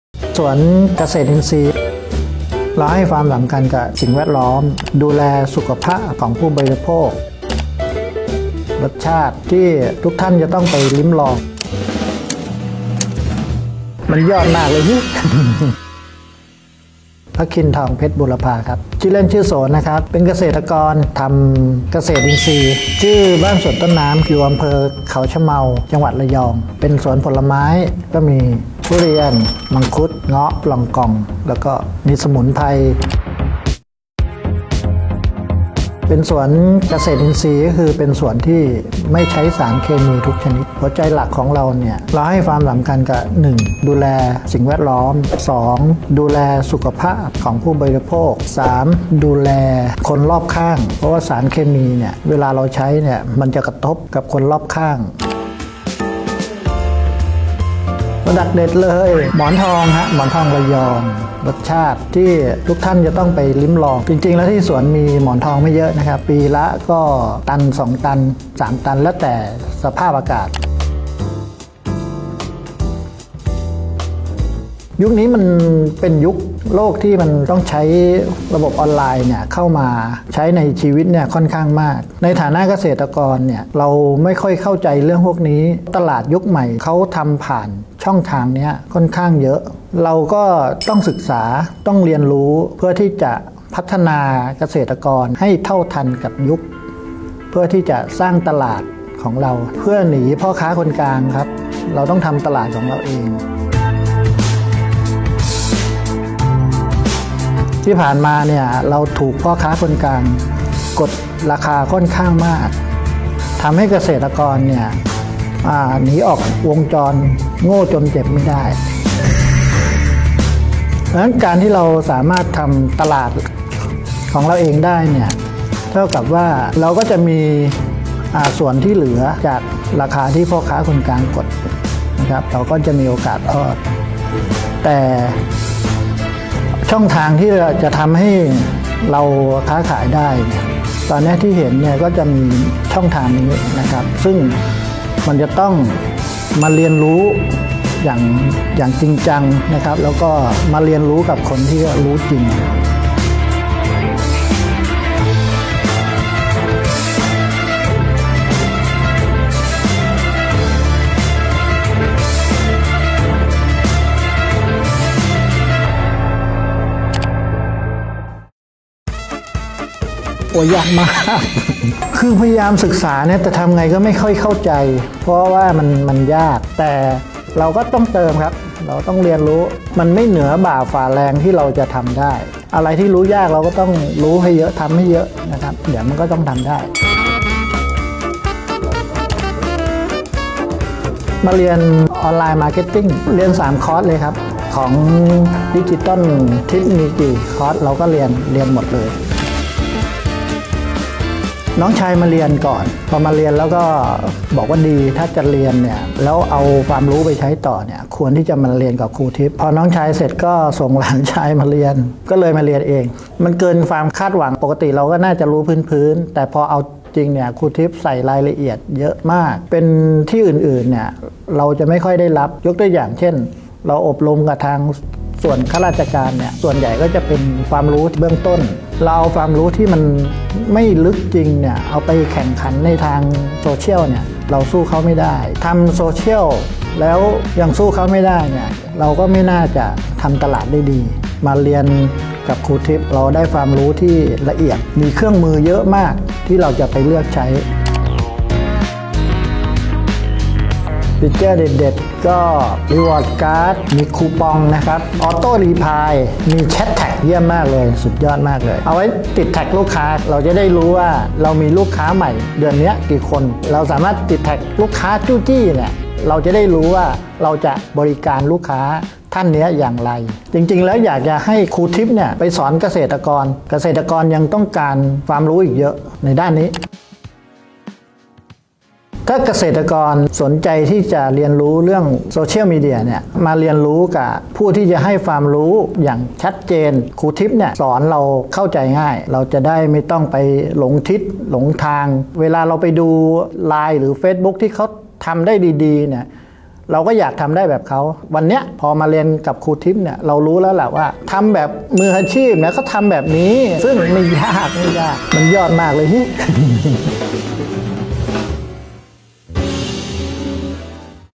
ทำไม? เกษตรกร ต้อง "ออนไลน์" พี่โส ภคิน ทองเพชรบูรพา | Digital Tips
🌳เกษตรกร ก็ต้อง "ออนไลน์" 🌳
และยุคต่อจากนี้ ไม่ว่าธุรกิจไหนก็ต้องก้าวสู่โลกออนไลน์!

มาฟังเสียงจากเกษตรกรตัวจริง ✅
พี่โส ภคิน ทองเพชรบูรพา จากจังหวัดระยองกันครับว่า

"ทำไมการทำออนไลน์ ถึงสำคัญกับชีวิตของพี่น้องเกษตรกรยุคต่อไป?"

ฟังแล้วเอาใจเชียร์สุดๆ เดินทางไปกลับ ระยอง - กรุงเทพฯ
เพื่อมาเรียนรู้ด้านดิจิทัล
✅ ที่สำคัญเรียนครบ 3 คอร์ส แบบจัดเต็มไปเลยจ้า ✅

ขอให้พี่โสเอาความรู้ไปปรับใช้กับสวนเกษตรอินทรีย์เยอะๆ เลยนะค้า

เลิฟๆๆๆ ค่า

ฝากติดตามช่อง DigitalTipsAcademy ด้วยนะค้า :)
แล้วคุณจะไม่พลาด สาระความรู้ใหม่ๆ เกี่ยวกับ Digital จากเราแน่นอน!